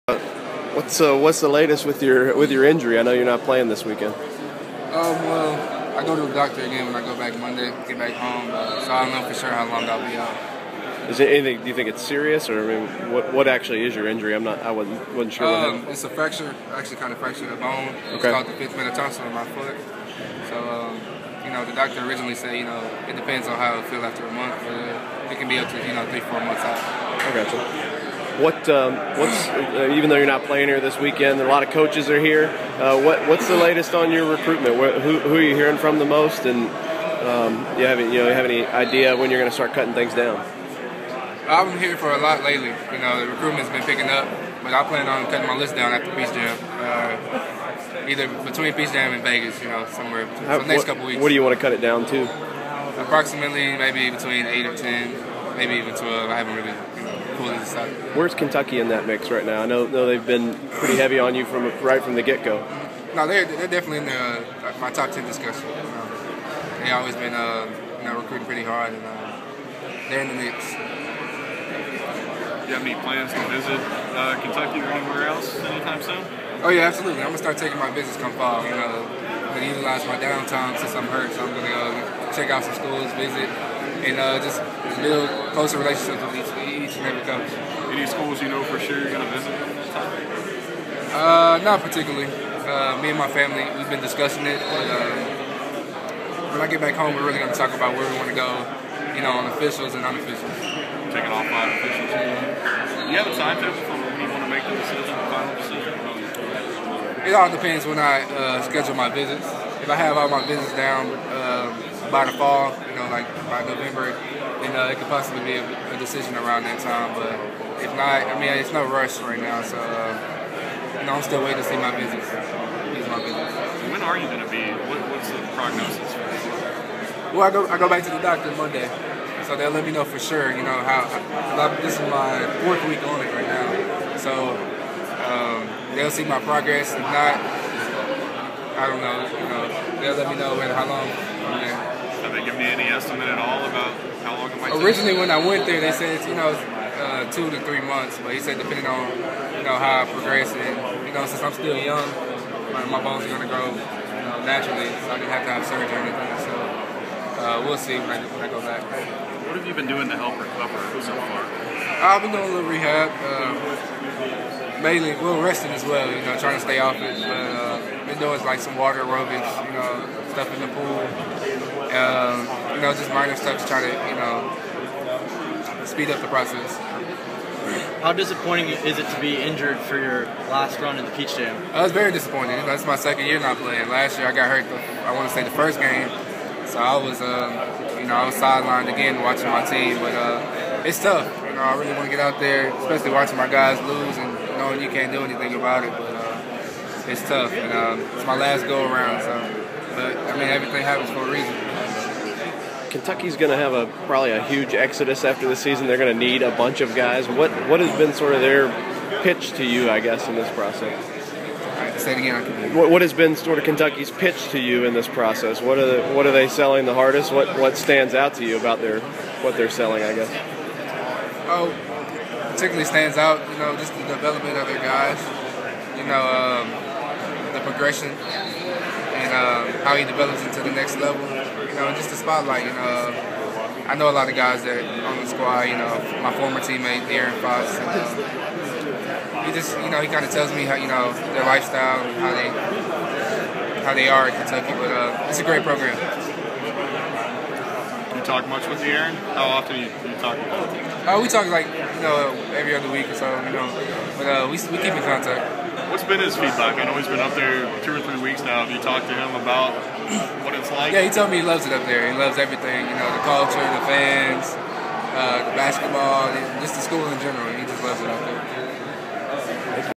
Peach Jam | 5-star PF Jarred Vanderbilt talks Kentucky, foot injury
Vanderbilt plans to cut his list to 10 schools soon, says the Cats are "in the mix."